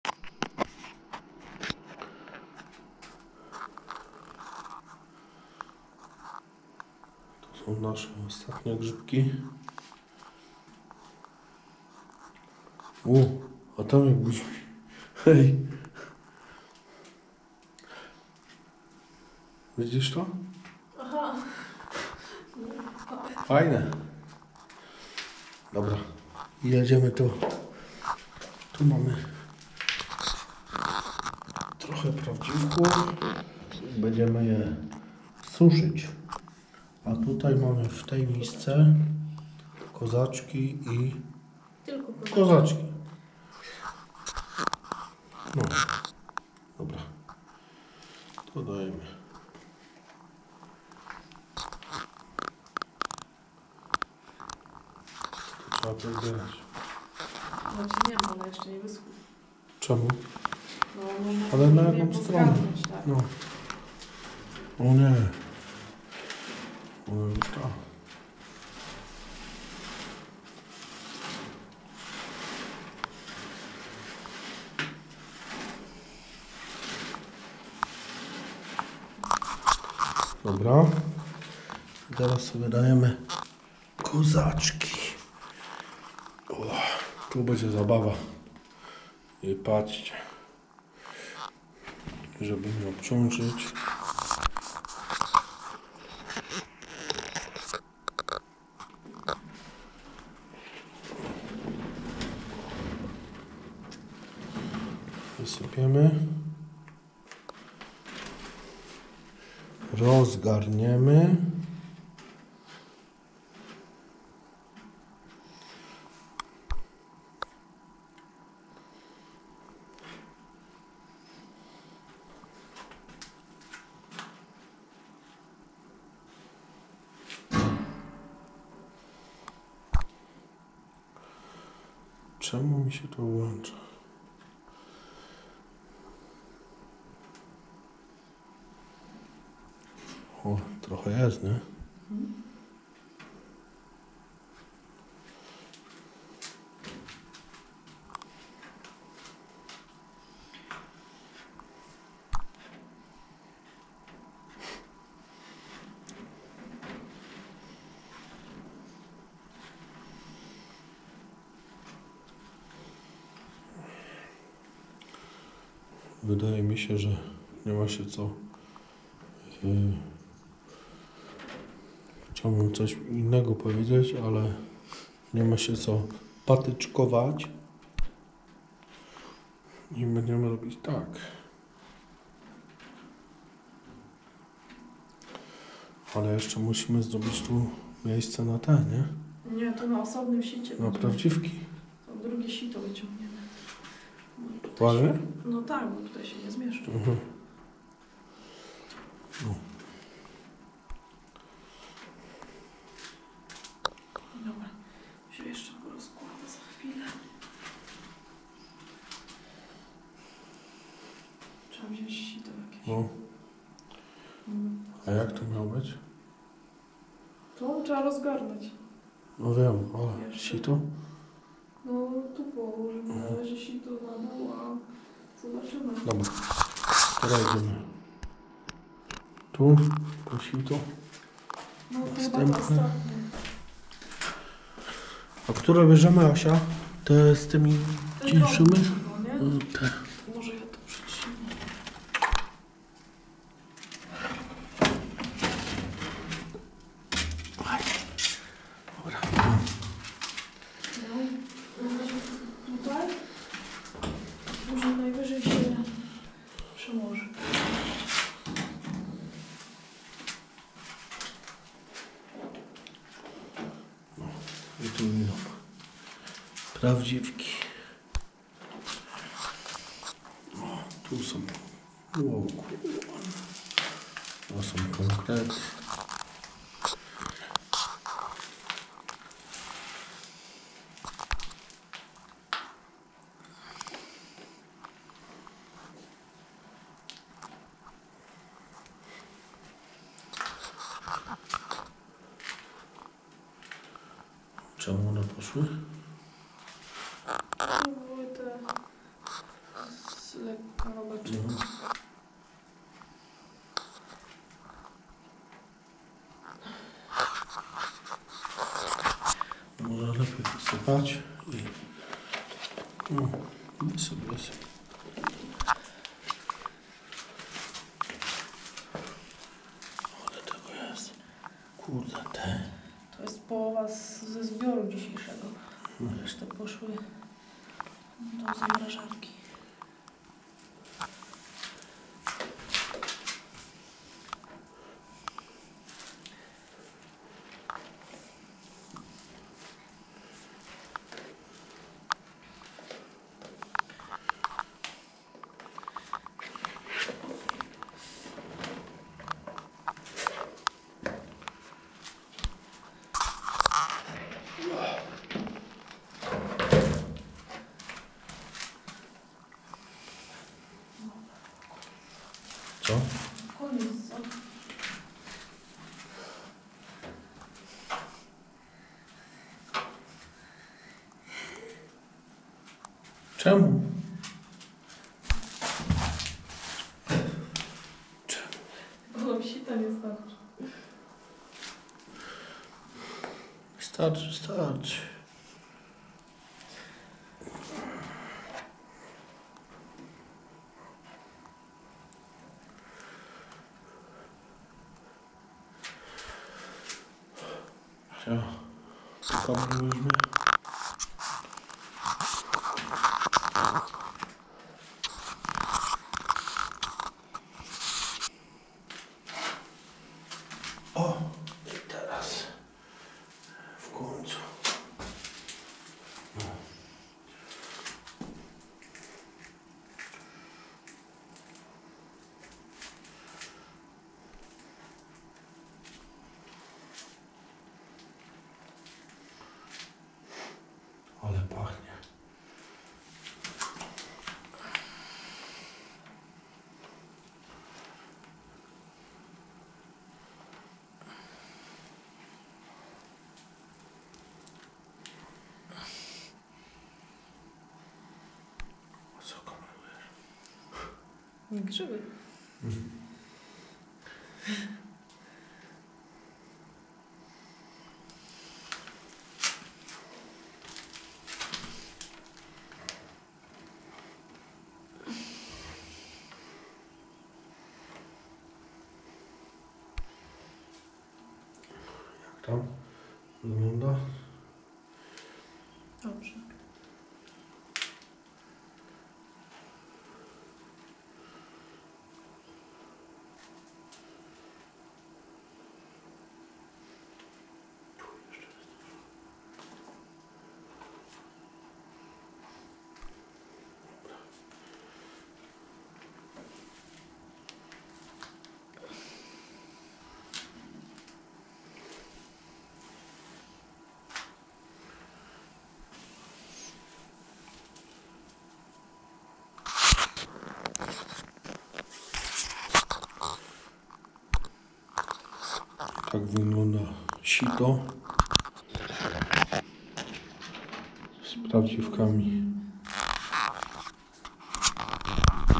Suszenie prawdziwków.
Nareszcie sito zapełnione i to nie byle czym tylko borowikami szlachetnymi.